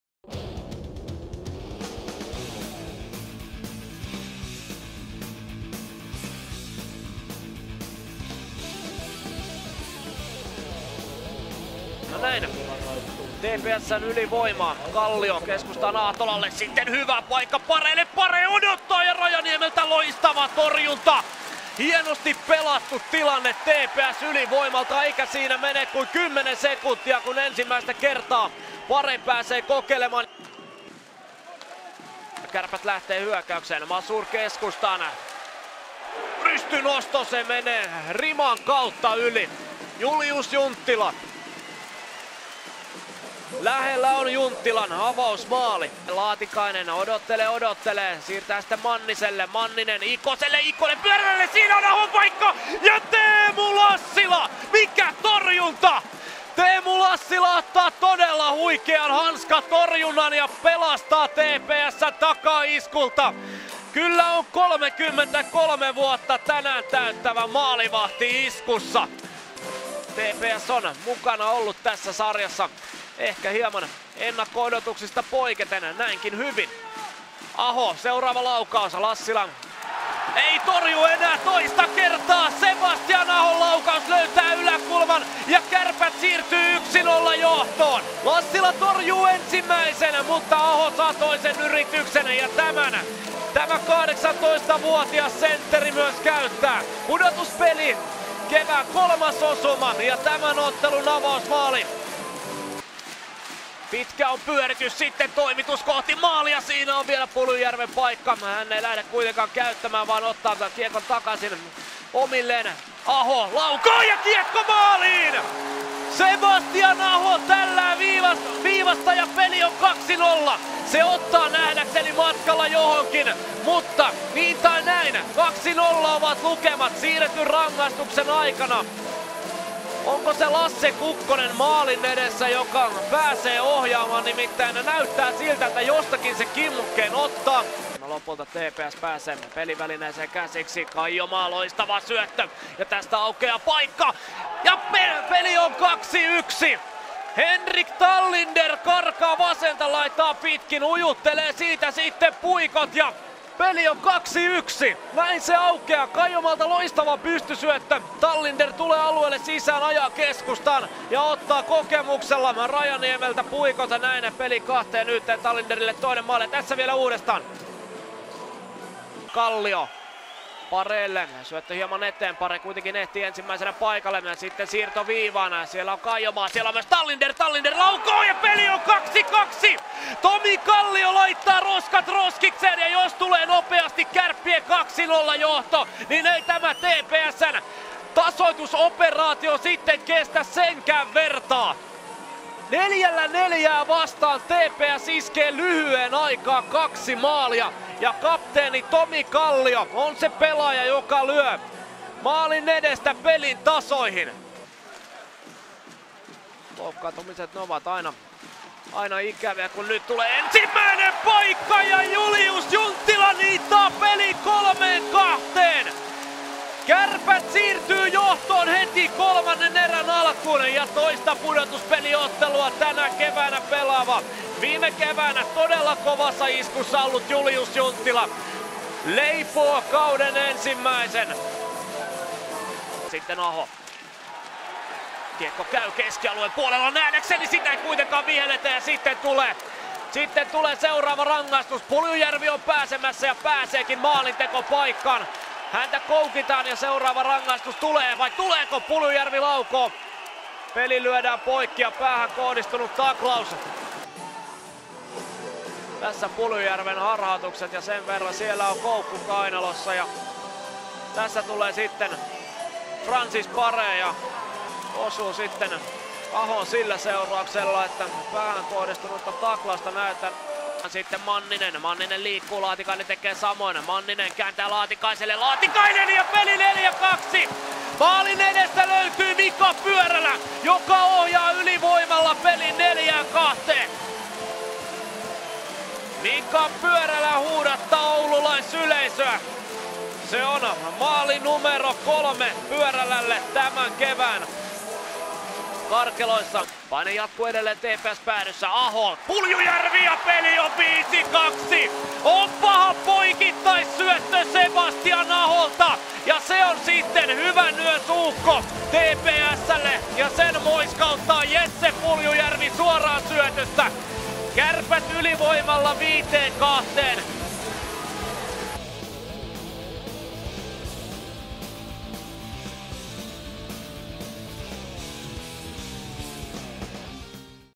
67. KÄRPÄT - TPS PLAYOFF 7 2015-2016 26.3.2016
MAALIKOOSTE JÄÄKIEKON VIIDENNESTÄ  PUOLIVÄLIERÄOTTELUSTA TPS - KÄRPÄT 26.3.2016